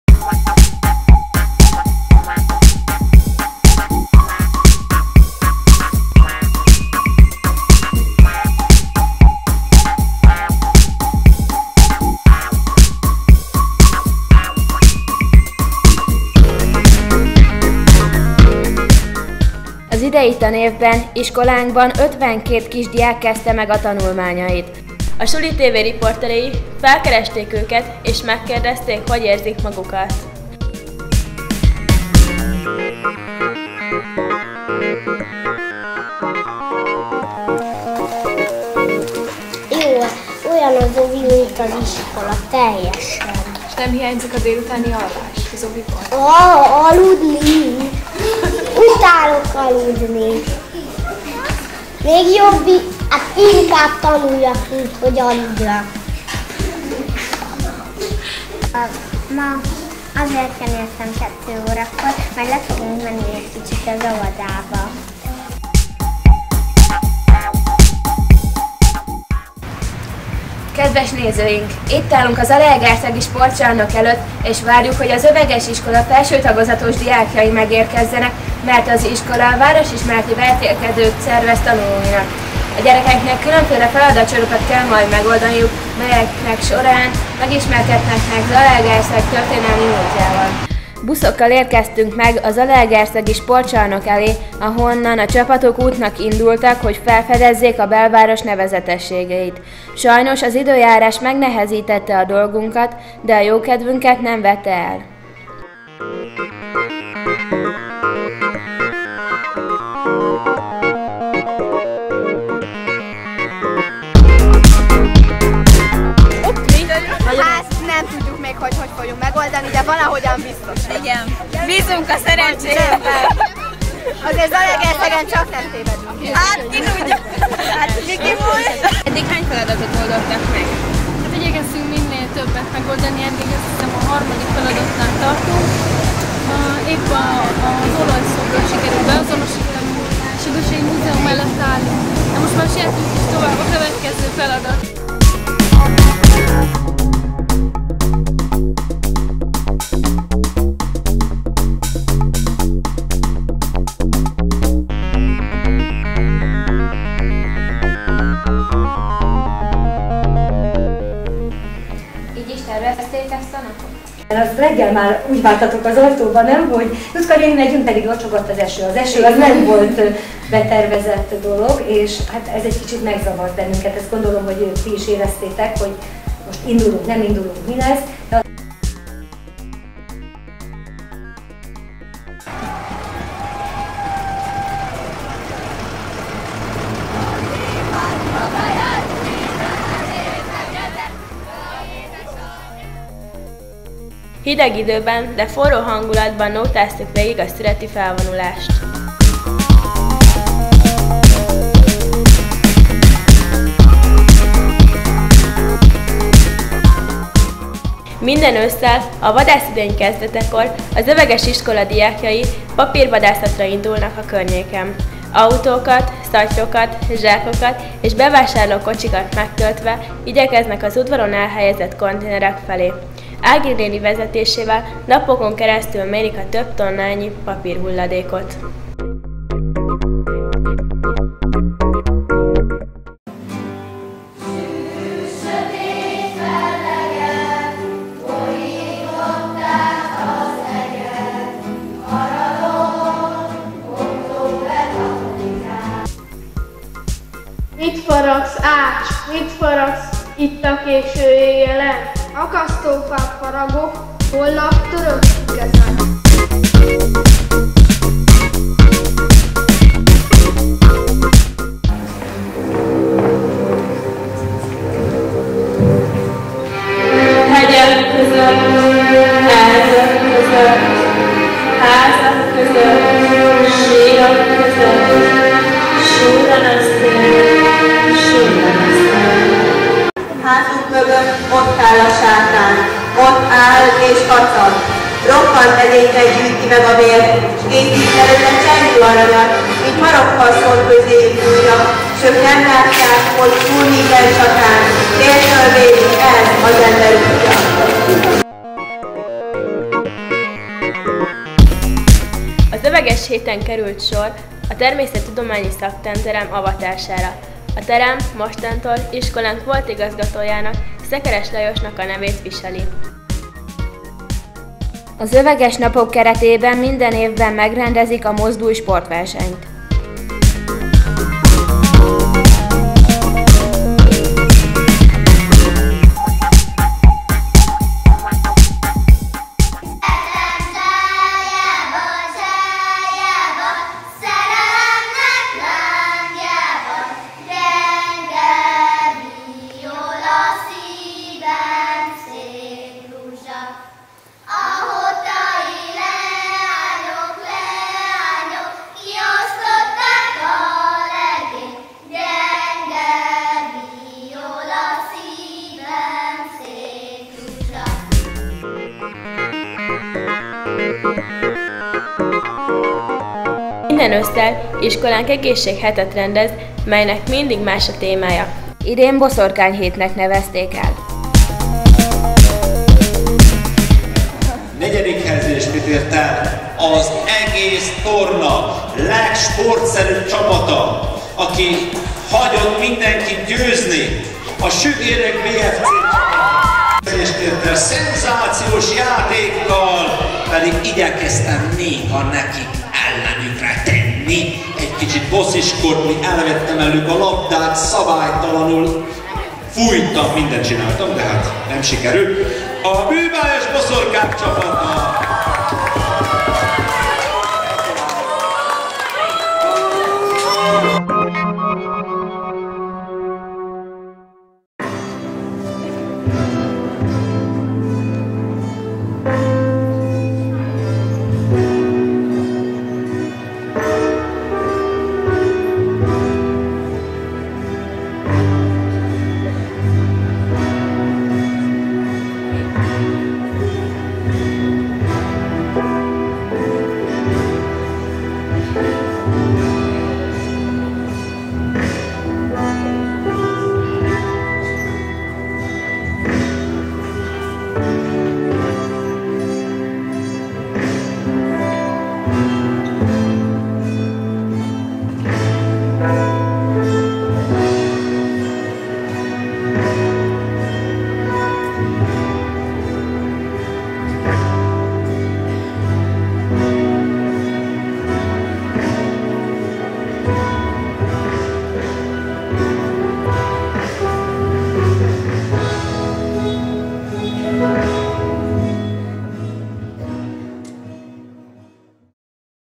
Öveges ÁMK Suli TV 1. adás
A zalaegerszegi Öveges József Általános  Művelődési Központ nyolcadikos tanulói készítették ezt a filmet. A Suli TV Baráti Kör a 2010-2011-es tanév első félévének eseményeit dolgozta fel rövid riportok formájában.